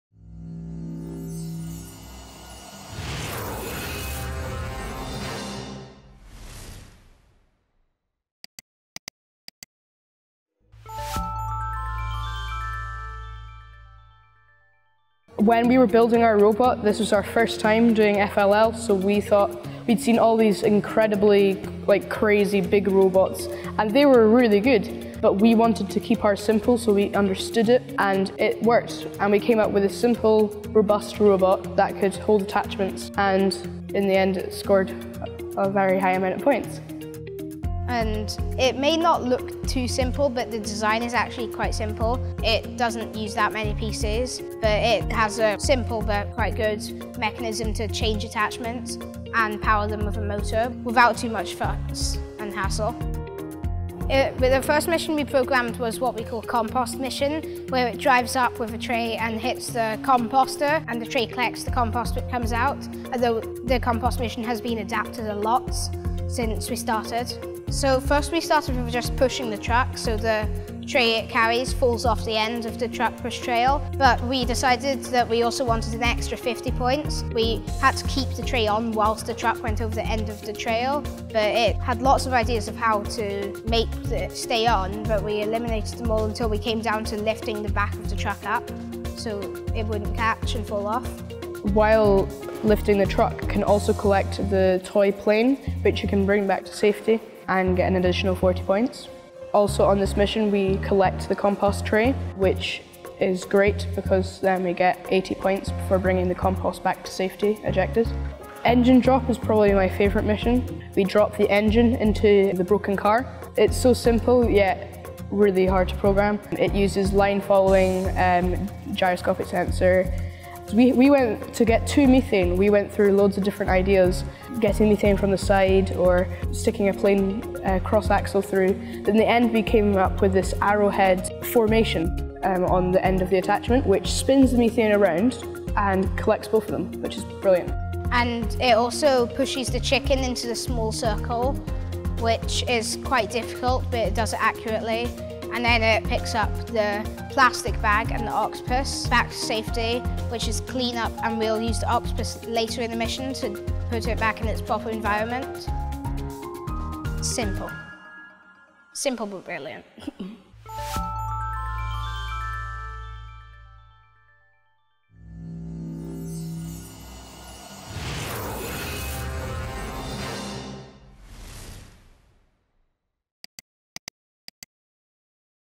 The West Linton Wasps' Robot
Check out West Linton Wasps' robot they used in the First LEGO League competition. With this robot, they went on to win the UK final.

FLL is for young people aged nine to 16 years, working in teams of up to ten with a supporting mentor. Each year FLL releases a new challenge for the teams.

The challenge involves a robot game and a project, and students will need to demonstrate the FLL core values throughout all their work.